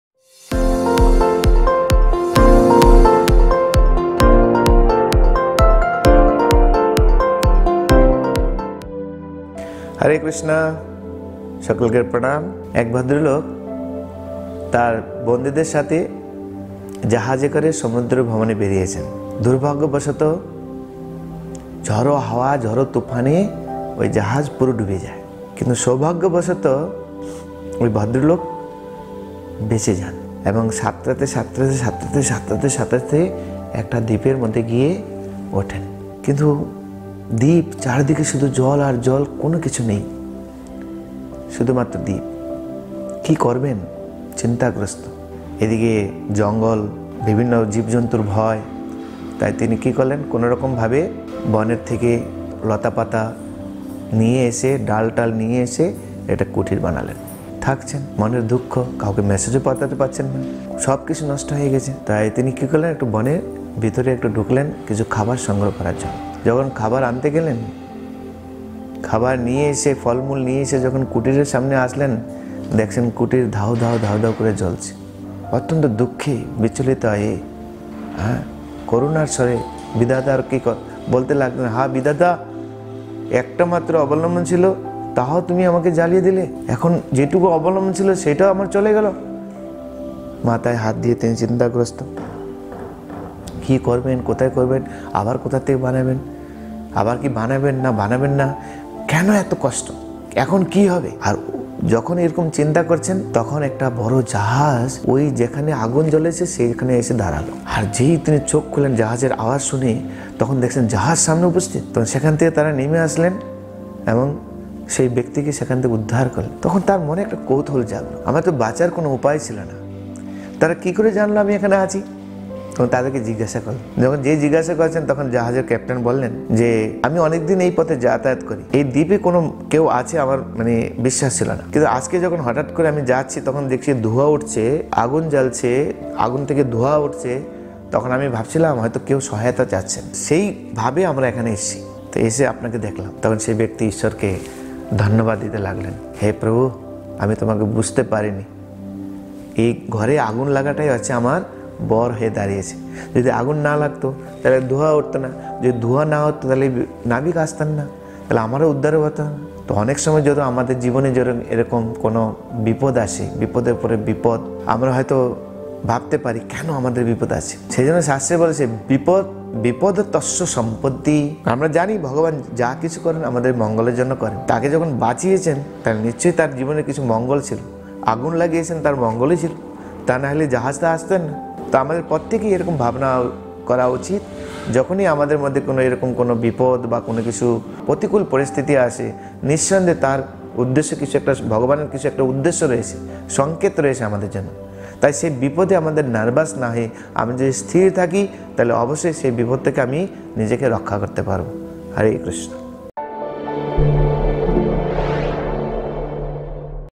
সৌভাগ্যের রহস্য || The secret of good luck || Prerona ( প্রেরণা ) || H G Lakshmi Gobinda Das
In this motivational video you will find  to success in life, this motivation video will help you achieve your goal and make your life successful.